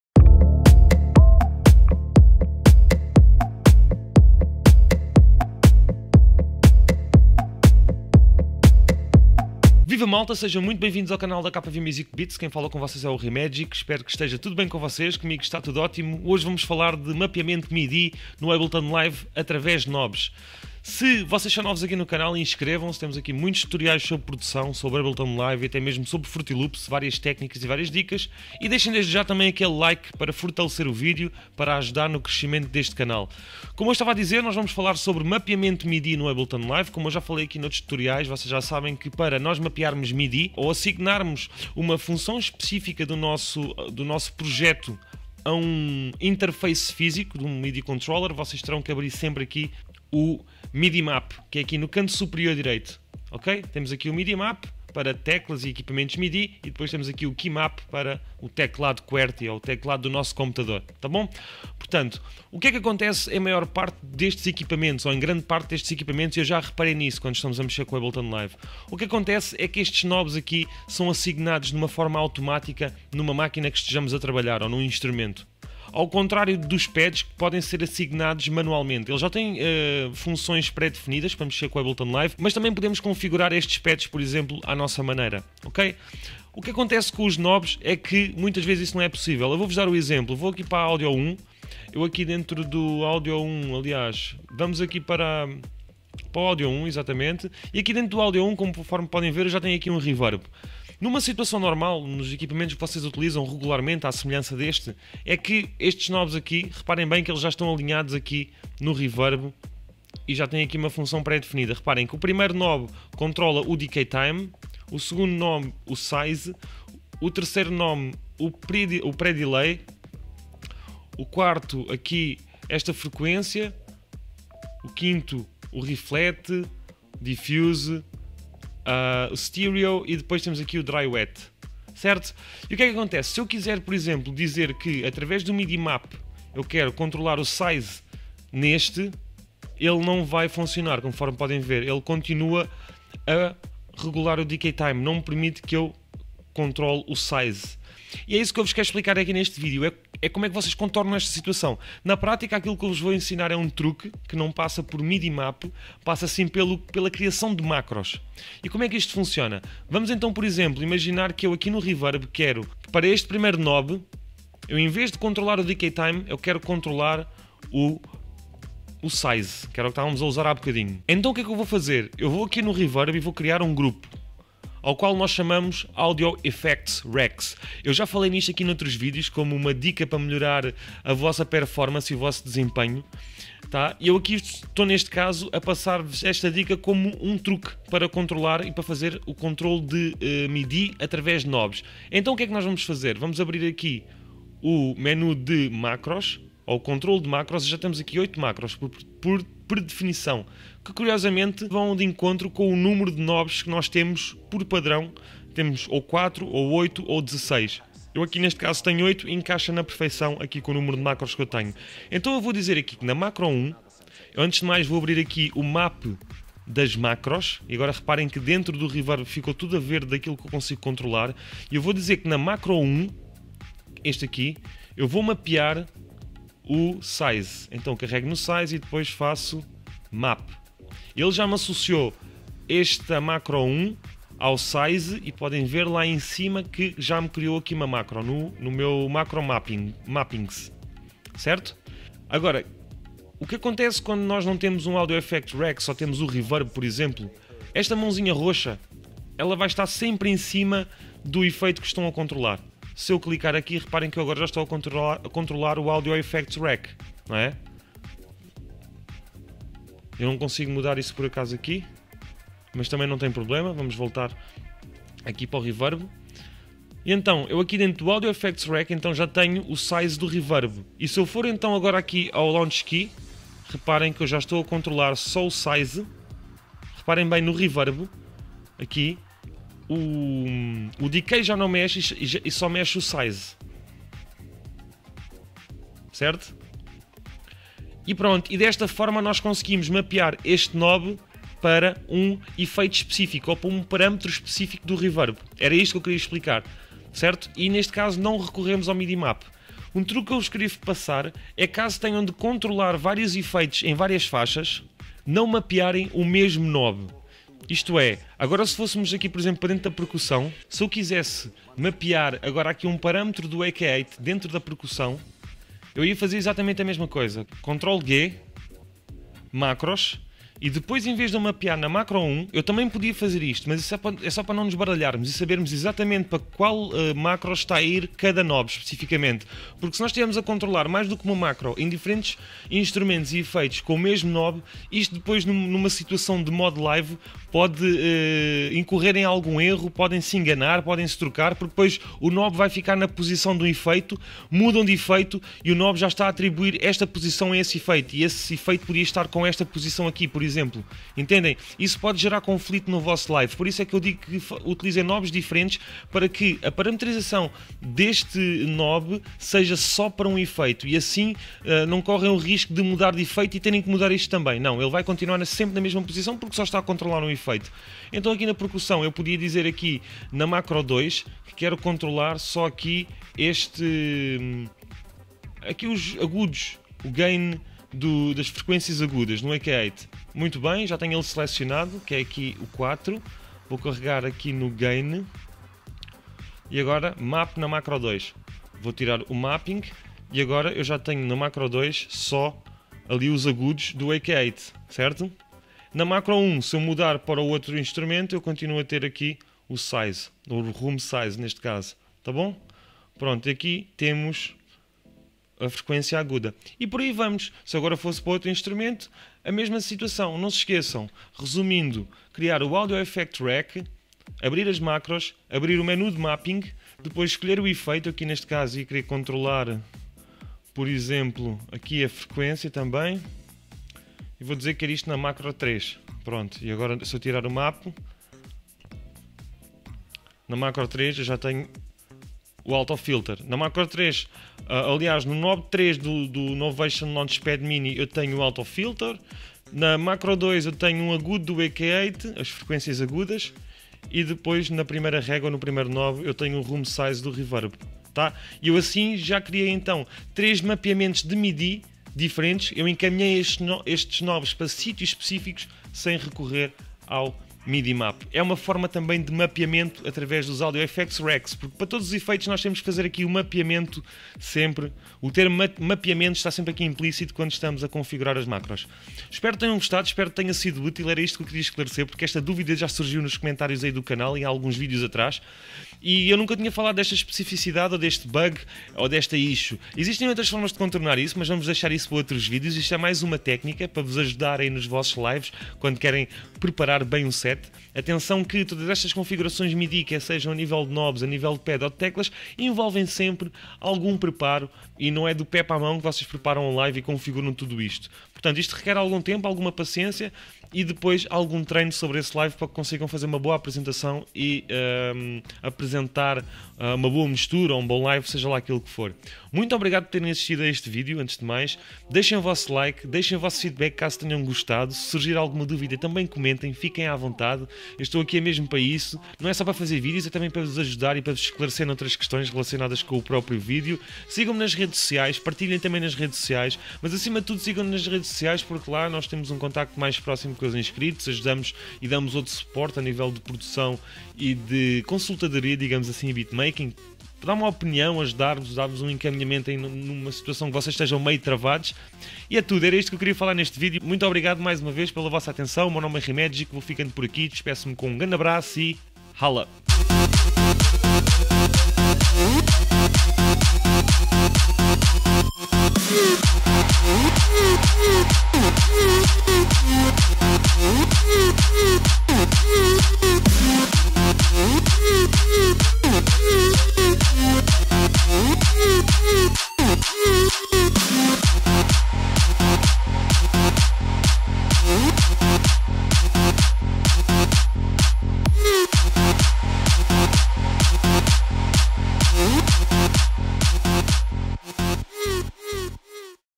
Ableton LIVE - Mapeamento KNOBS (Tutorial Avançado PT/BR)
Ableton LIVE - Mapeamento de KNOBS (Tutorial Avançado em Português). Como mapear os knobs do controlador MIDI através de macros nos Audio Effects Racks. Todas as semanas novos tutoriais de LIVE e Produção Musical e Vlogs. Goste ✔ Comente ✔ Partilhe ✔